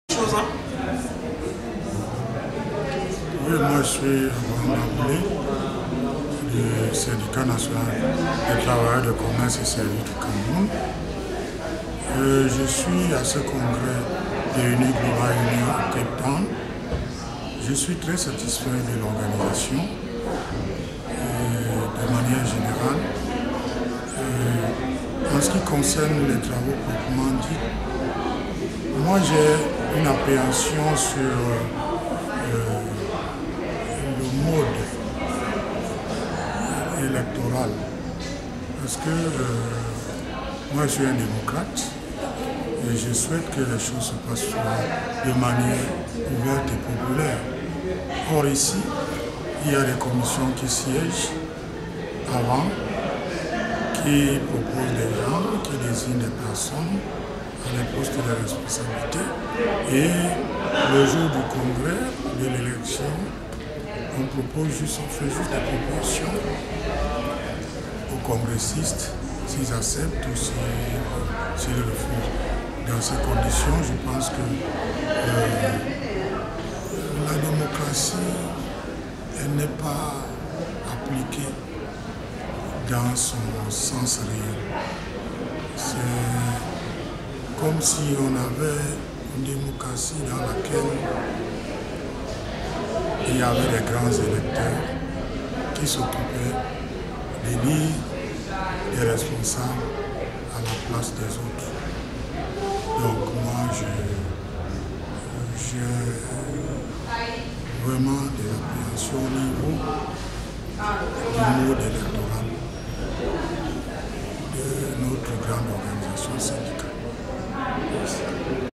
Rasoule Abraham - SYNATCOS
Rasoule Abraham speaks to Uniglobal 2014